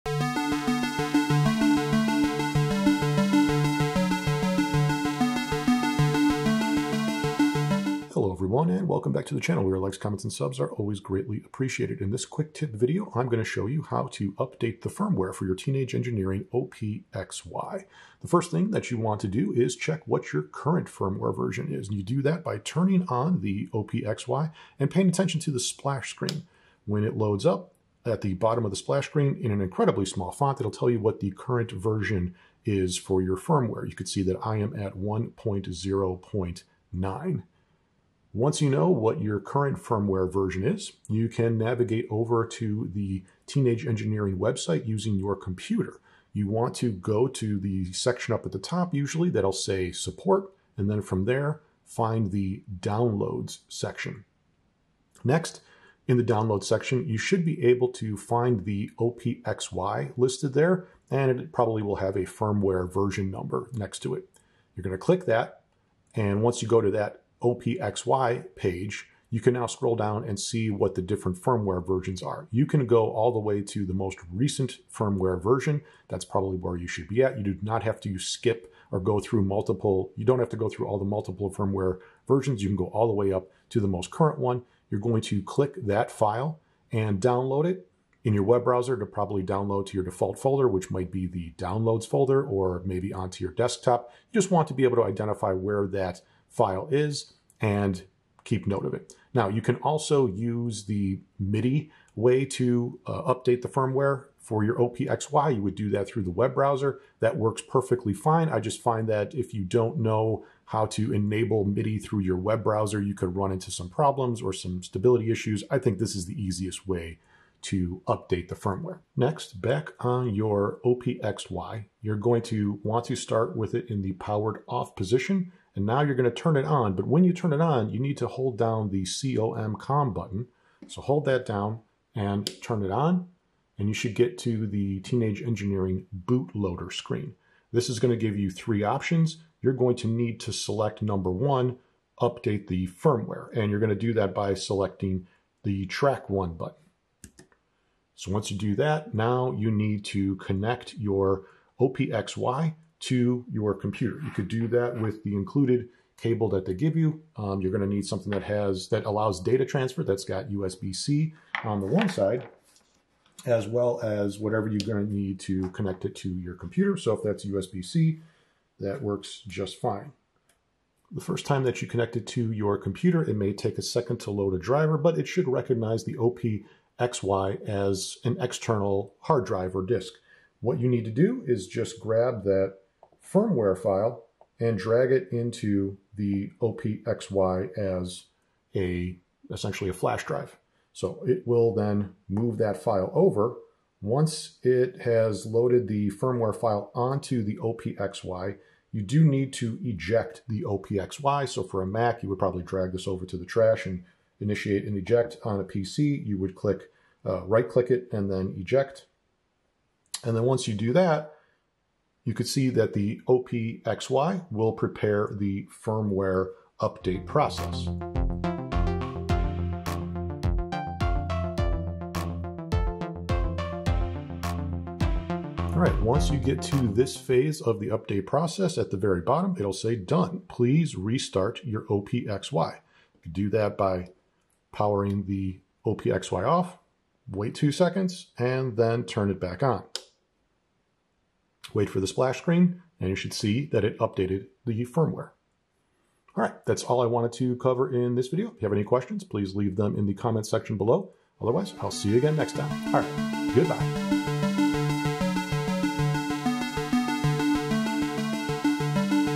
OP-XY: How to update the firmware
This Quick Tip tutorial will demonstrate an easy way to update the firmware of the Teenage Engineering OP-XY synth/sampler/sequencer.

Chapters
0:00 Intro
0:20 Current Firmware
0:42 Website Download
2:00 te Boot
2:30 Connect Computer
3:30 Update